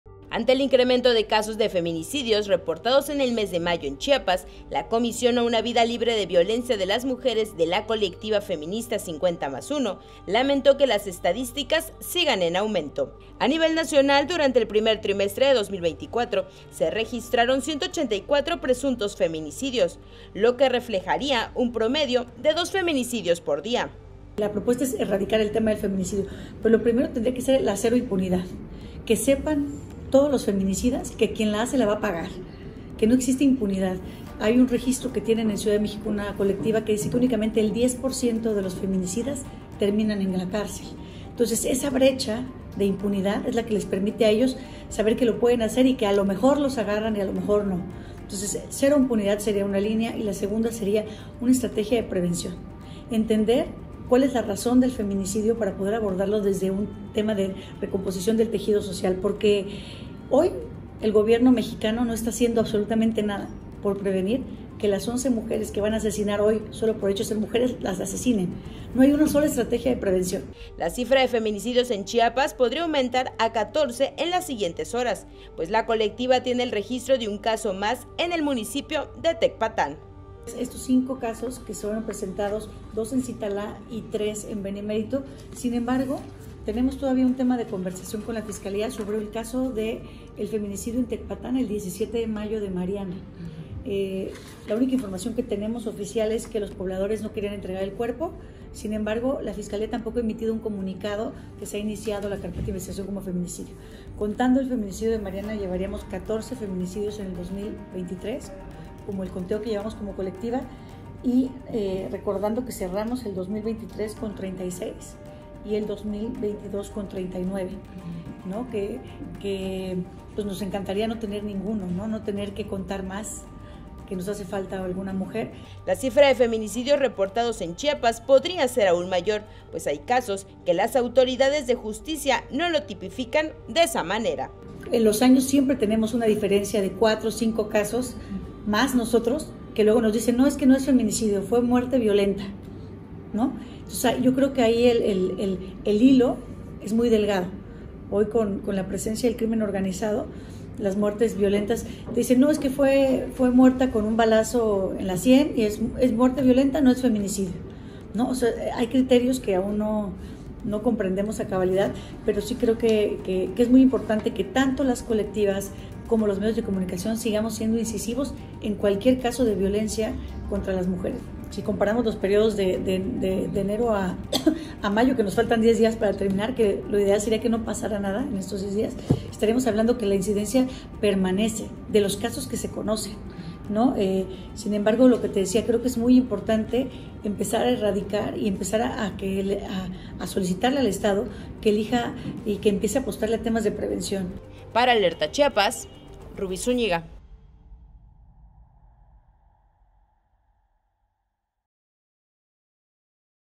Dos feminicidios por día, en México
En Chiapas han incrementado los feminicidios en Chiapas, esta semana un nuevo caso en Tecpatán.